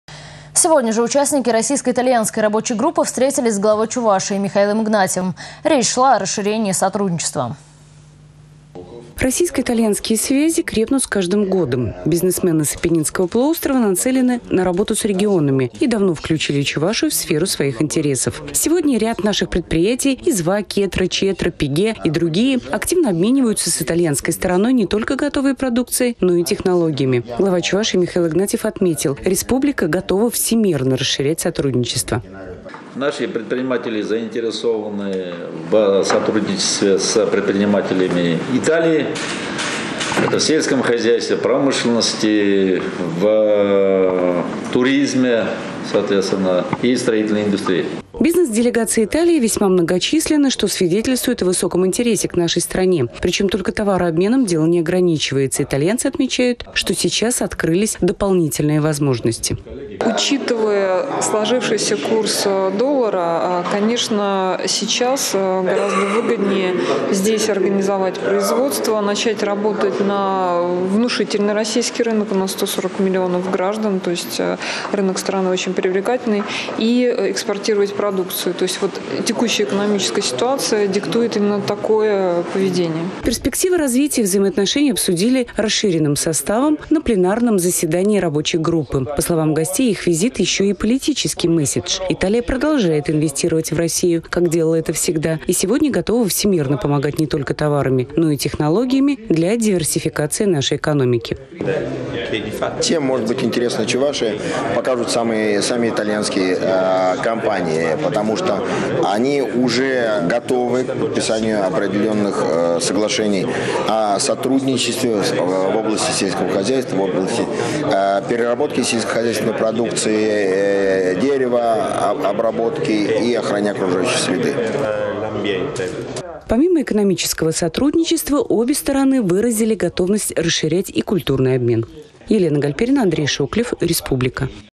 Встреча Главы Чувашии с итальянцами
Сегодня же участники российско-итальянской рабочей группы встретились с Главой Чувашии Михаилом Игнатьевым. Речь шла о расширении сотрудничества.